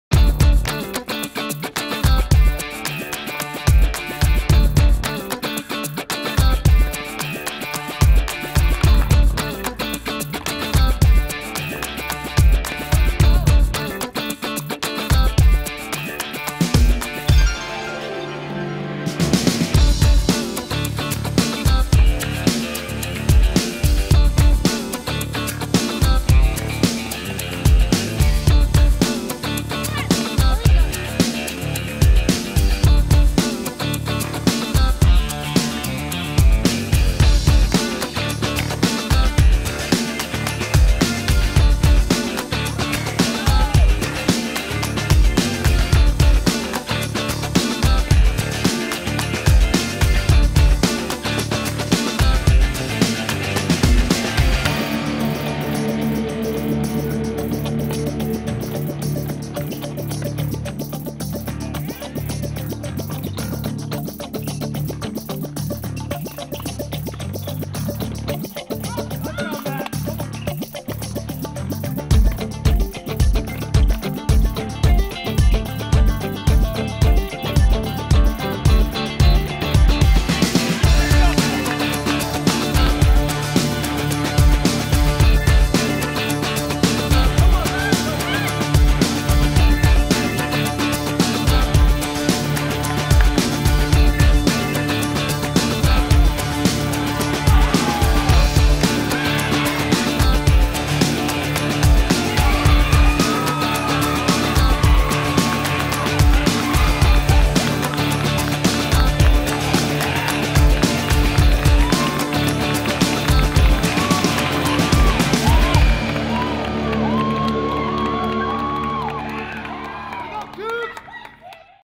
Women's Tennis - BYU vs Saint Mary's - April 21, 2018
Highlights from the Women's Tennis Win over Saint Mary's at the Outdoor Tennis Courts on Senior Day on April 21,2018